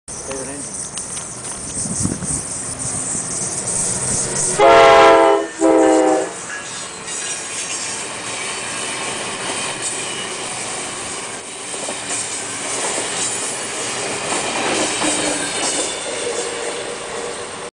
AmtrakNECFAN927 this goes out for u! HORN!!!!!!!!
A westbound regional at Nan interlocking came through. With a AEM-7 for power. I just learned over the internet that the AEM-7 is starting to go away. Oh by the way Horn!